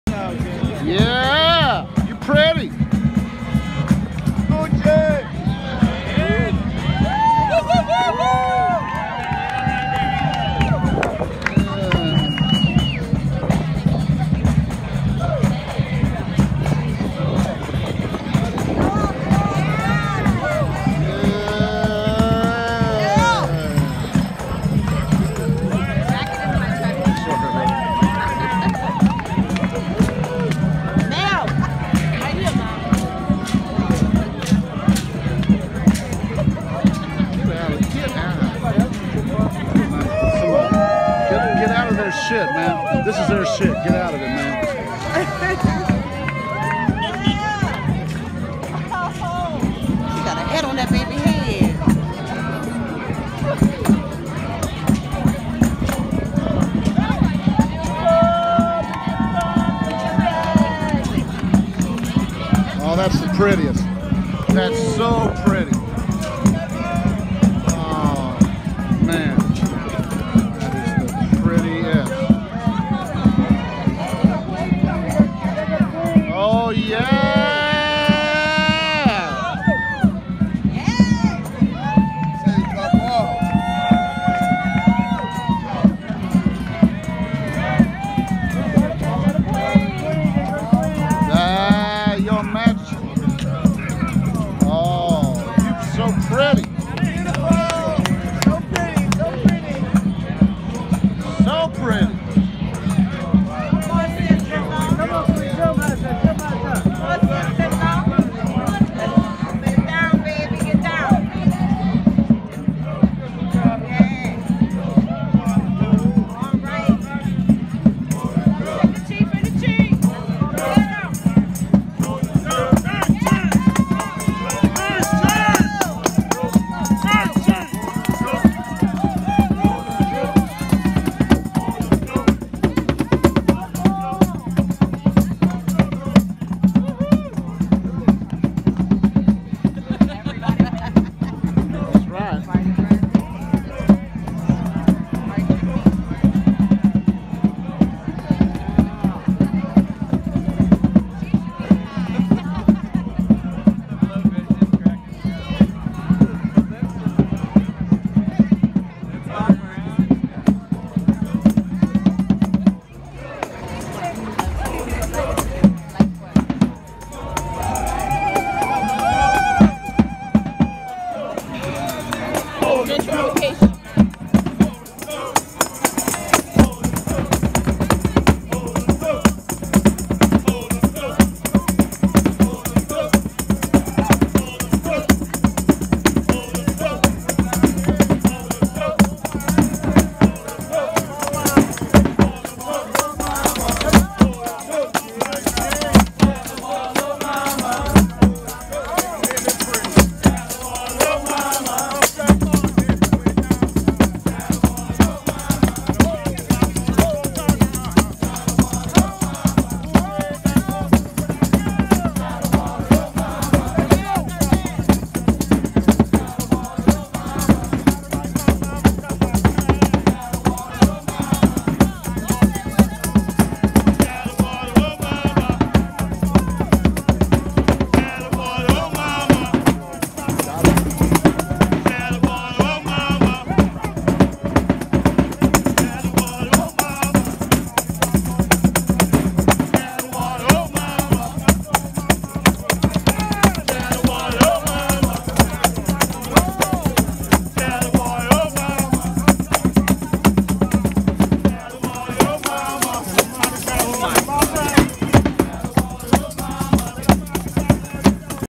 Entrance of the 9th Ward Black Hatchets Mardi Gras Indian Tribe, Mardi Gras Day, New Orleans, 2019
The entrance of the 7th Ward Black Hatchets starting with the Wild Man as I couldn't get my camera quick enough to catch the Spy Boy or the Flag Boy, who were very pretty. Amazing sewing, deep culture. Honored to witness.